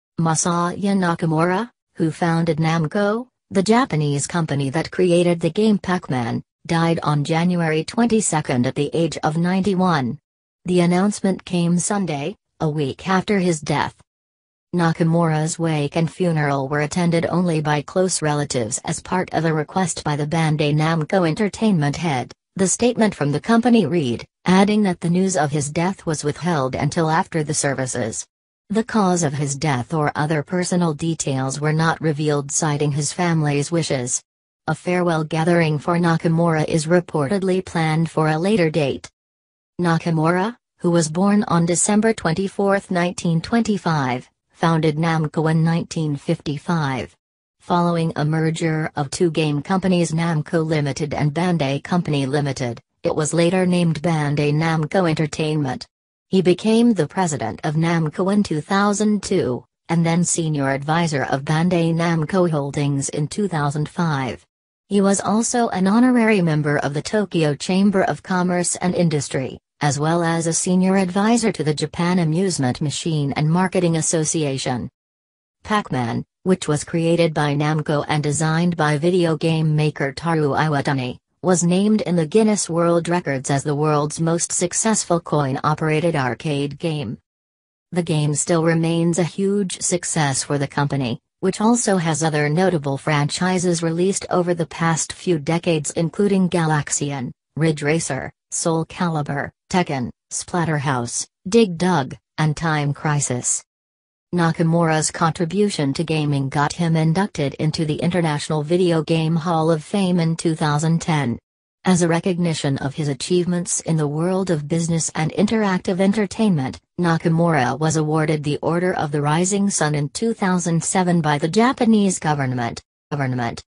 Who Is Masaya Nakamura ‘Father Of Pac Man’ And Founder Of Japanese Company Namco Dies Aged 91
Masaya Nakamura, who founded Namco, the Japanese company that created the game Pac-Man, died on Jan. 22 at the age of 91. The announcement came Sunday, a week after his death.

Nakamura's wake and funeral were attended only by close relatives as part of a request by the Bandai Namco Entertainment head, the statement from the company read, adding that the news of his death was withheld until after the services. The cause of his death or other personal details were not revealed citing his family's wishes. A “Farewell Gathering” for Nakamura is reportedly planned for a later date.

Nakamura, who was born on Dec. 24, 1925, founded Namco in 1955. Following a merger of two game companies Namco Ltd. and Bandai Co. Ltd., it was later named Bandai Namco Entertainment. He became the president of Namco in 2002, and then senior adviser of Bandai Namco Holdings in 2005. He was also an honorary member of the Tokyo Chamber of Commerce and Industry, as well as a senior adviser to the Japan Amusement Machine and Marketing Association.
Pac-Man, which was created by Namco and designed by video game maker Toru Iwatani, was named in the Guinness World Records as the world’s most successful coin-operated arcade game.

The game still remains a huge success for the company, which also has other notable franchises released over the past few decades including Galaxian, Ridge Racer, Soul Calibur, Tekken, Splatterhouse, Dig Dug and Time Crisis.

Nakamura's contribution to gaming got him inducted into the International Video Game Hall of Fame in 2010. As a recognition of his achievements in the world of business and interactive entertainment, Nakamura was awarded the Order of the Rising Sun in 2007 by the Japanese government.